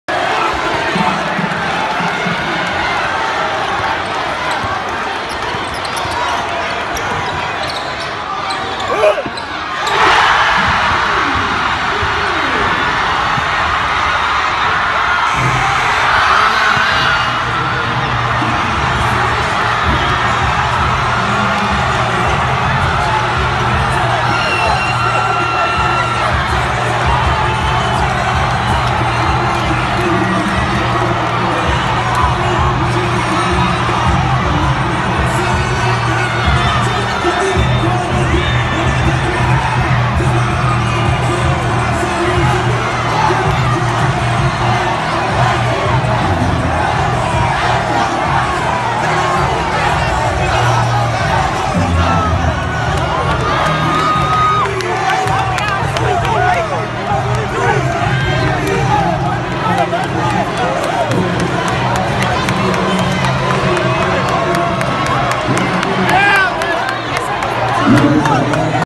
Martaveous McKnight makes game-winning shot for UAPB against Jackson State
UAPB junior guard Martaveous McKnight makes a game-winning shot with 2.4 seconds left to beat Jackson State 60-58, completing a 19-point comeback. The crowd storms the court.
The Golden Lions (8-15, 8-1 SWAC) remains atop the Southwestern Athletic Conference standings. Jackson State falls to 9-13, 6-3.